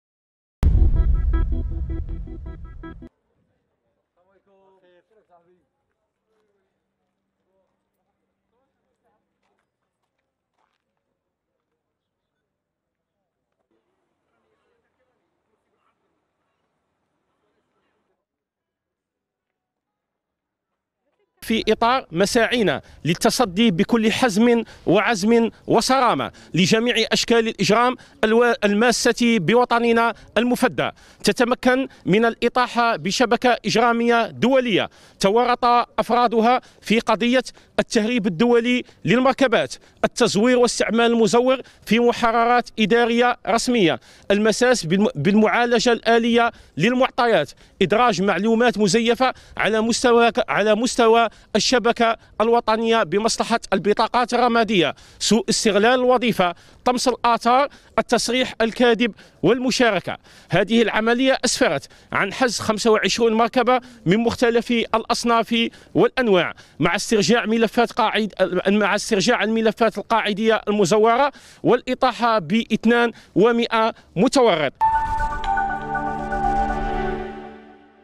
وهران: تفكيك شبكة إجرامية متورطة في قضية التهريب الدولي للمركبات
وهران (وأج) - تمكنت مصالح الشرطة لوهران من تفكيك شبكة إجرامية متورطة في قضية تخص التهريب الدولي للمركبات تضم 102 شخصا حسبما علم لدى مصالح الأمن الولائي.